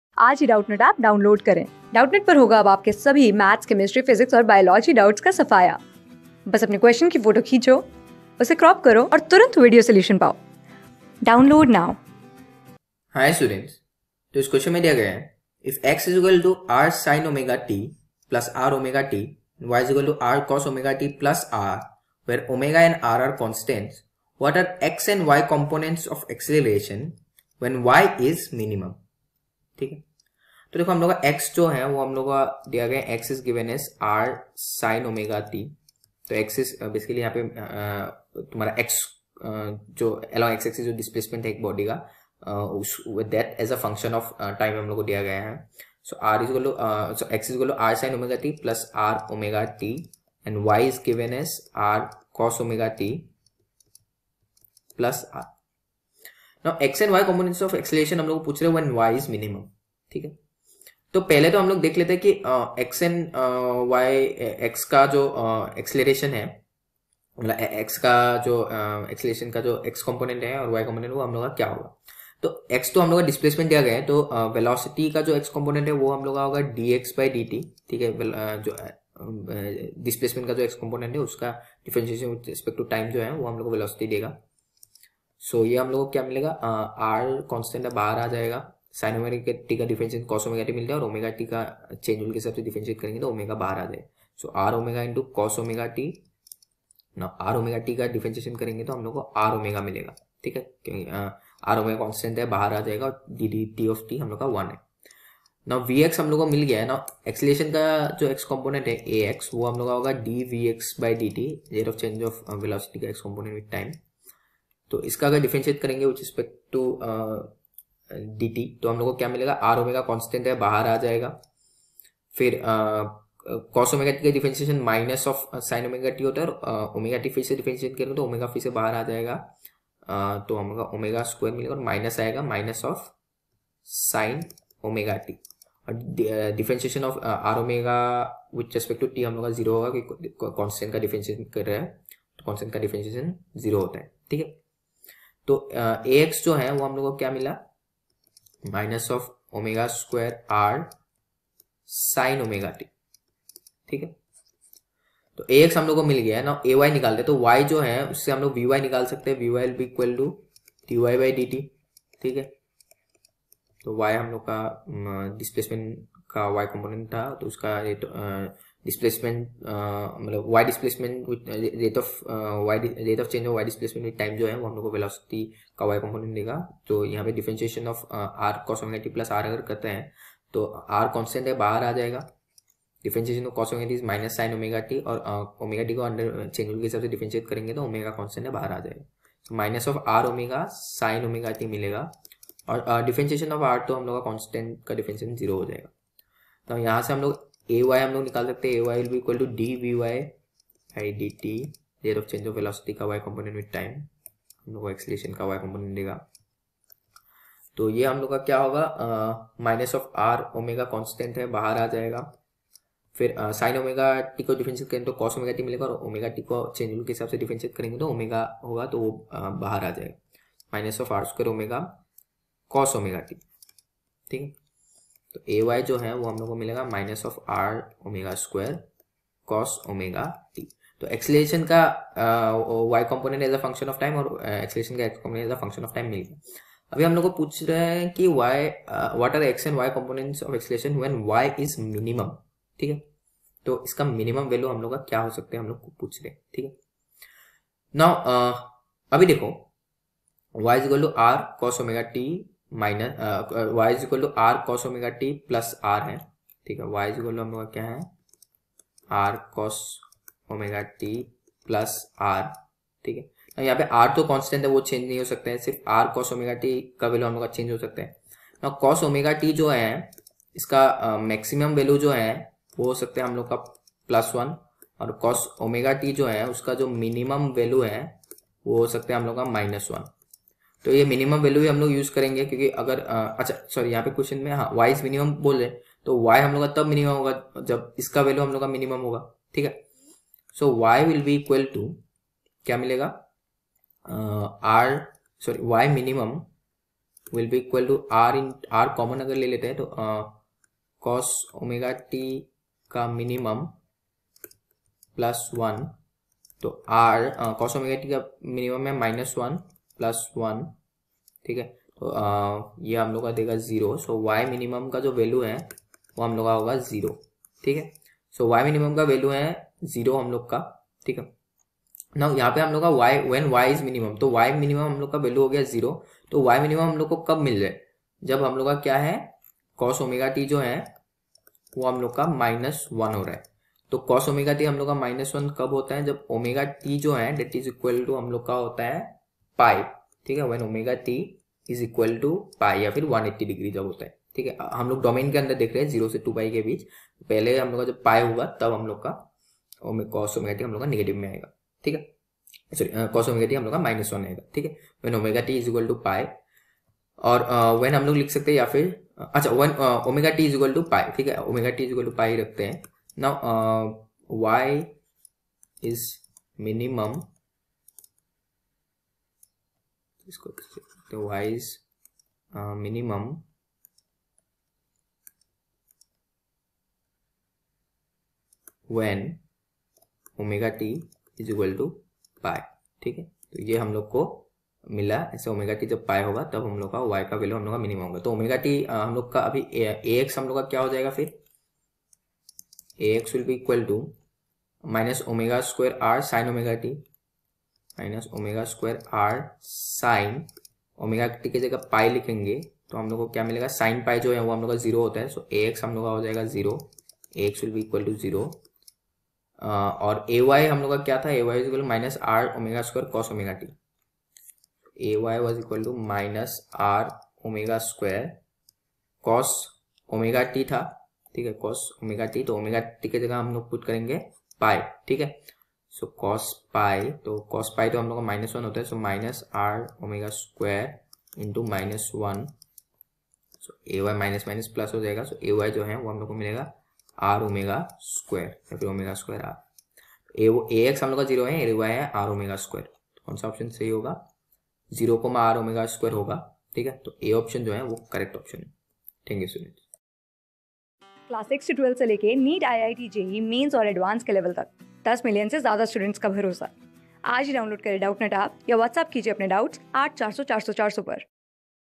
If x = R sin omega t + R omega t and y = R cos(omega t) + R (where omega and R are constants), w...
If x = R sin omega t + R omega t and y = R cos(omega t) + R (where omega and R are constants), what are x and y components of acceleration when y is minimum ?
Class: 11
Subject: PHYSICS
Chapter: OSCILLATIONS
Board:IIT JEE

👉 Have Any Query? Ask Us.